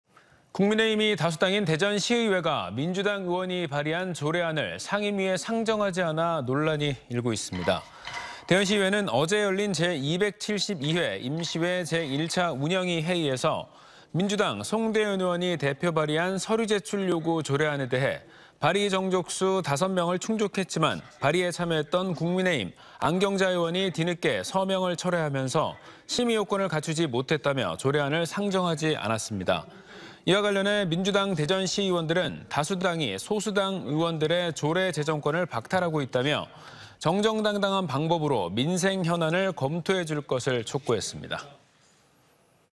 대전시의회, 민주당 발의 조례안 상임위 미상정 논란｜ TJB 대전·세종·충남뉴스
국민의힘이 다수당인 대전시의회가
민주당 의원이 발의한 조례안을
상임위에 상정하지 않아
논란이 일고 있습니다.

대전시의회는 어제 열린 제272회 임시회
제1차 운영위 회의에서 민주당 송대윤 의원이 대표발의한 '서류제출 요구 조례안'에 대해
발의 정종수 5명은 충족했지만, 발의에
참여했던 국민의힘 안경자 의원이 뒤늦게
서명을 철회하면서 심의요건이 안된다는 이유로 조례안을 상정하지 않았습니다.

이와 관련해 민주당 대전시의원들은 다수당이 소수당 의원의 조례 제정권을 박탈하고 있다며 정정당당한 방법으로 민생현안을 검토해 줄 것을 촉구했습니다.